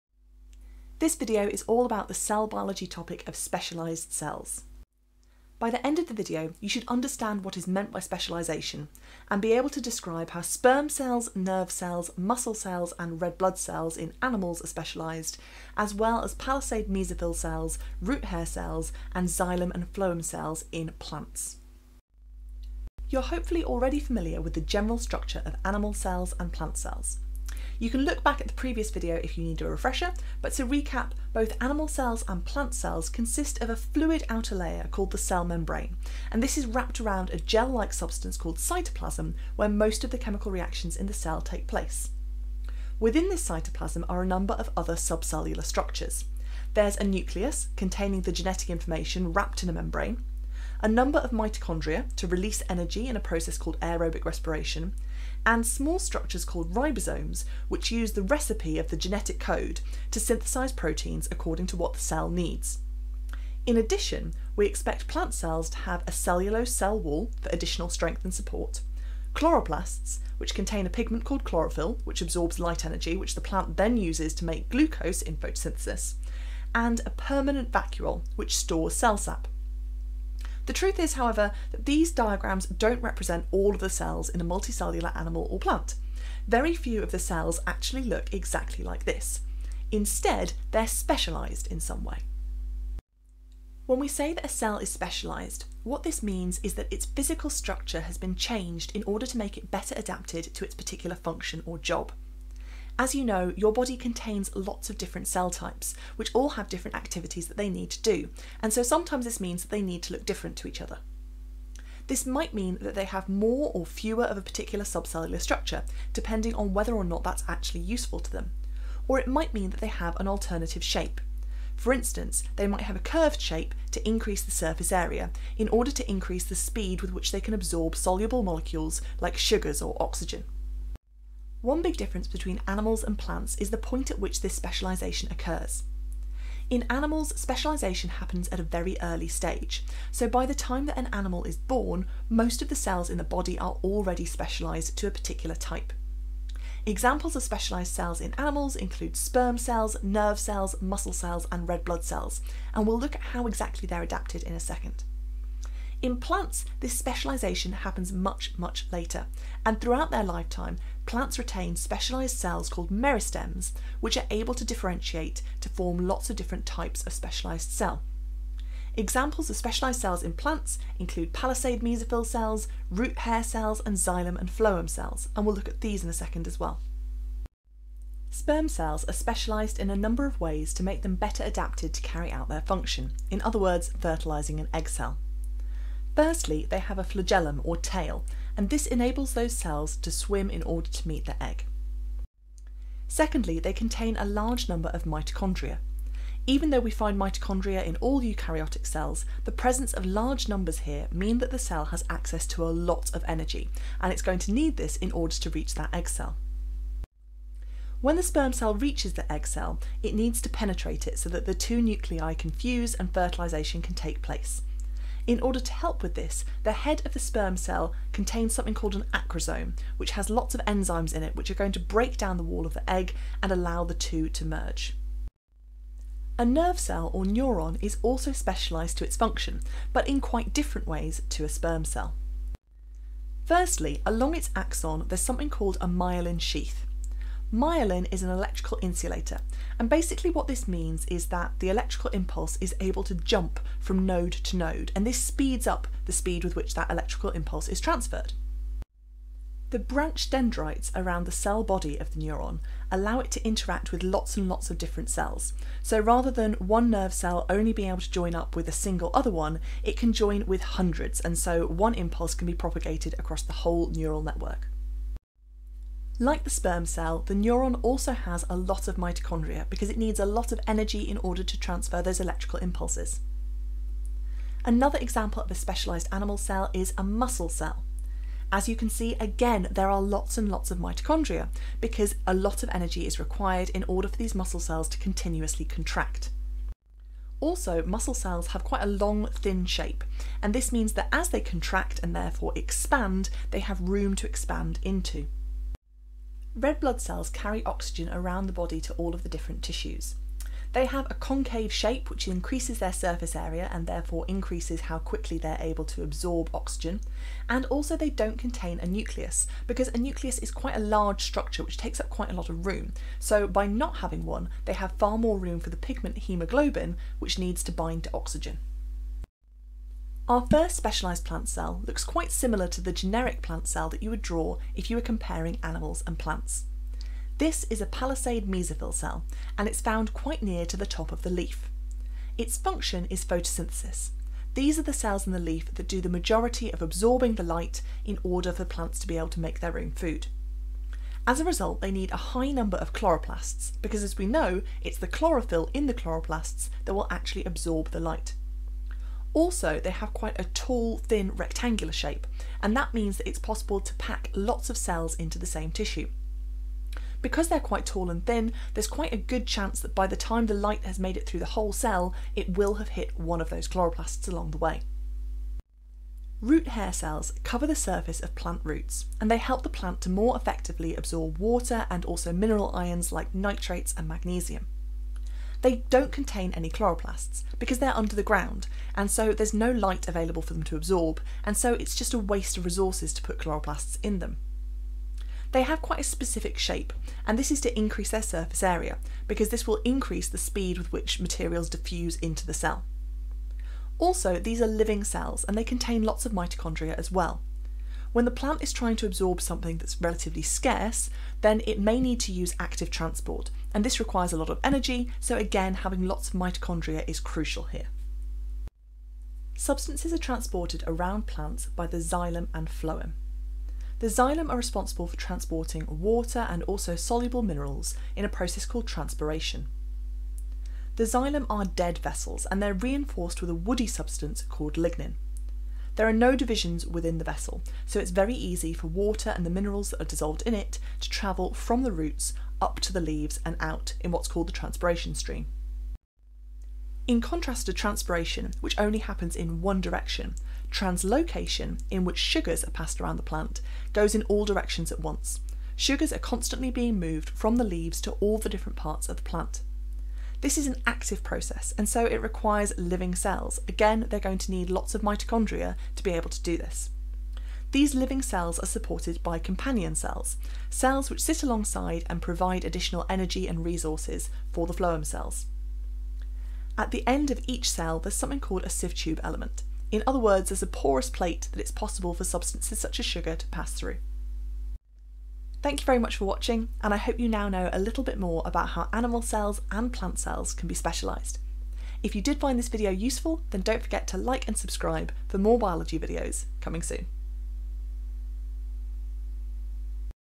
Specialised Cells
A revision video mainly aimed at students studying for AQA GCSE Biology (8461) or GCSE Combined Science (8464) describing the specialisation of animal and plant cells. (See specification part 4.1.1.3 Cell specialisation)